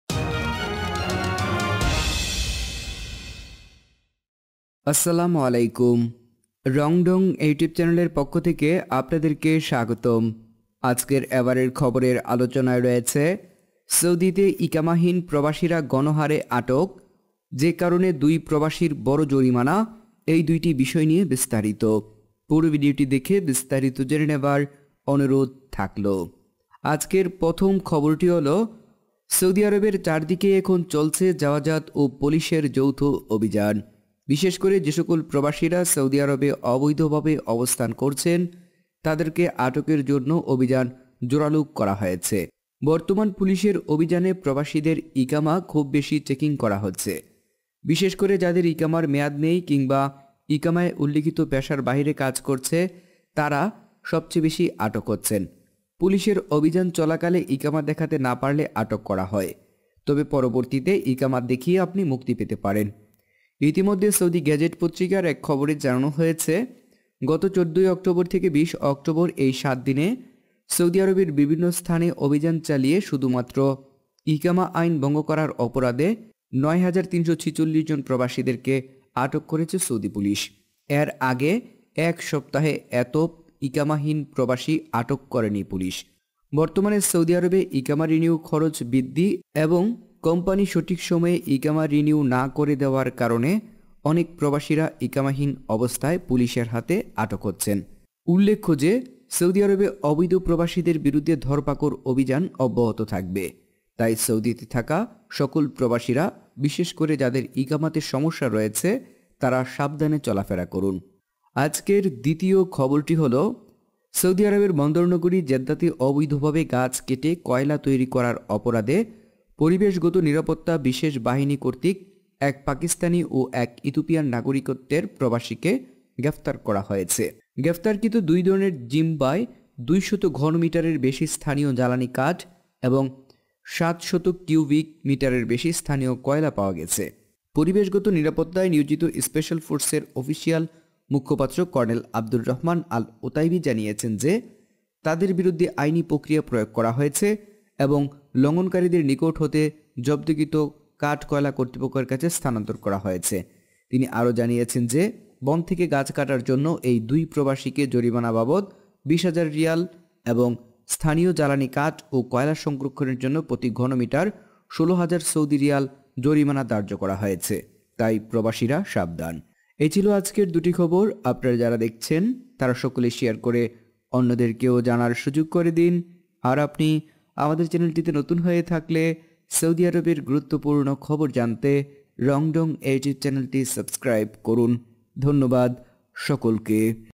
সৌদিতে ইকামাহীন প্রবাসীরা গন হারে আটক | যে কারণে ২ প্রবাসীর বড় জরিমানা
আজকের এবারের খবরে আলোচনায়  রয়েছে সৌদিতে ইকামাহীন প্রবাসীরা গন হারে আটক ।  যে কারণে ২ প্রবাসীর বড় জরিমানা  সেই বিষয়ে বিস্তারিত। পুরো ভিডিওটি দেখে বিস্তারিত জেনে নেবার অনুরোধ রইলো।